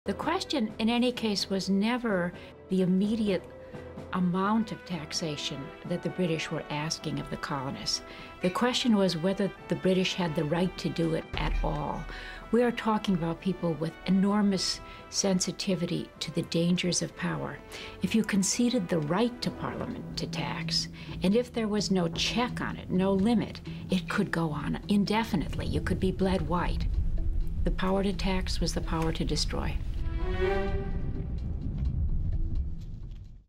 The Power of Taxation
"The power to tax is the power to destroy," says Historian Pauline Maier regarding the Stamp Act and the implication it held for the status of British colonists in America. The worry not only was "taxation without representation," but also "taxation without limitation" by Parliament.  Today, in the US, we definitely have representation, but paired with a worrisome tendency to ignore any limitations. The fundamental question must always be, does government serve the people, or people the government?